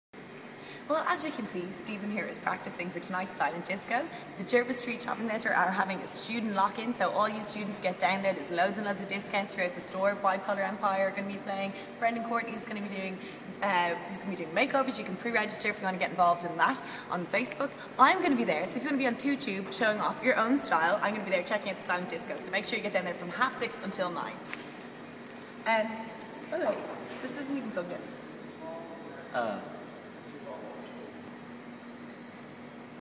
Join The Student Lock-In
Join Bláthnad tonight at the Student Lock-In at the Jervis Street Shopping Centre for the best silent disco ever along with shop discounts, make-overs AND Bláthnad will be chatting with everyone about their style.

If you pre-register on facebook you will be automatically registered for the chance to get a makeover by stylist Brendan Courtney!

Get down there for 6:30pm it's on until 9pm  Don't worry, the fun won't stop there as there's an after party too!